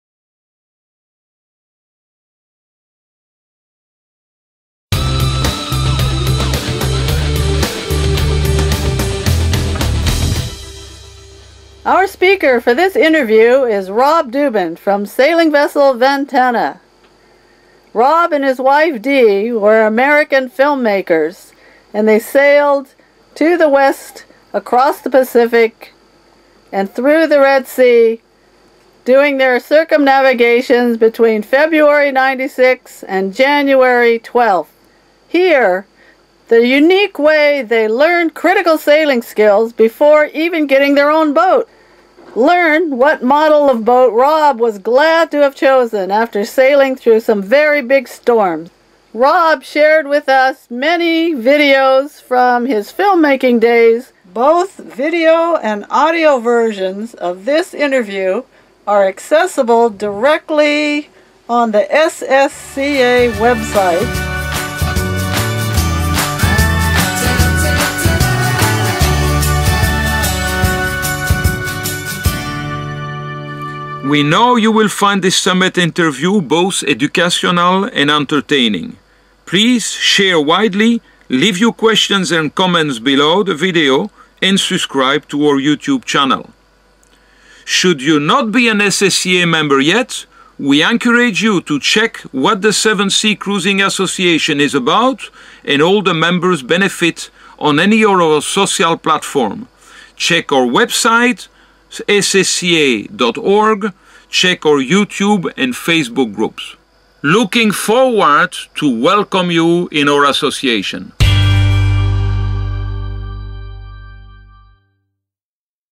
SSCA Circumnavigators Preview - Rob Dubin sv Ventana
A sneak preview of the Circumnavigators Summit interview of Rob Dubin.
This 6th interview in the Series will also be accessible on ssca.org (Publications menu) from September 4 to coincide with the discovery of the island of Manhattan by navigator Henry Hudson.